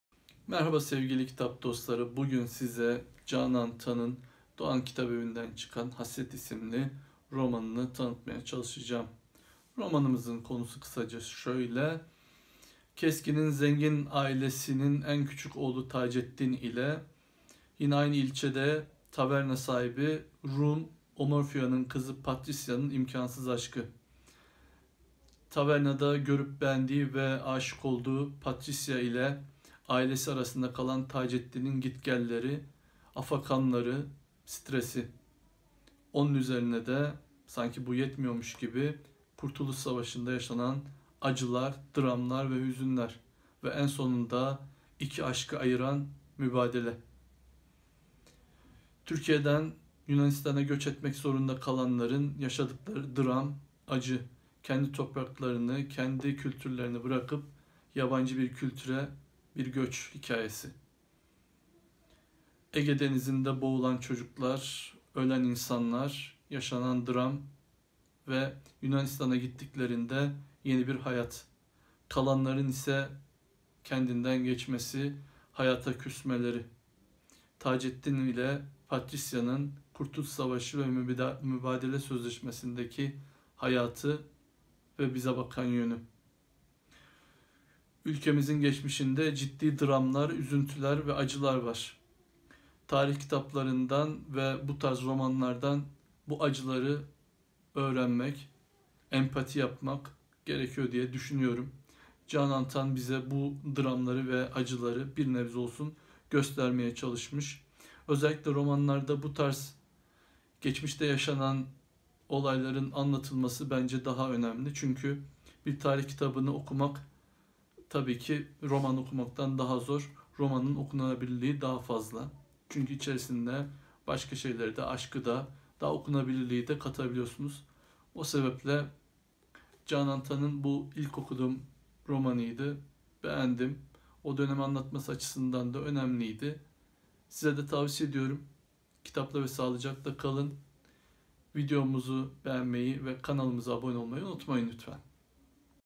KİTAP TANITIM CANAN TAN “HASRET”
Canan Tan’ın mübadele döneminde yaşanan imkansız aşkı anlatıyor.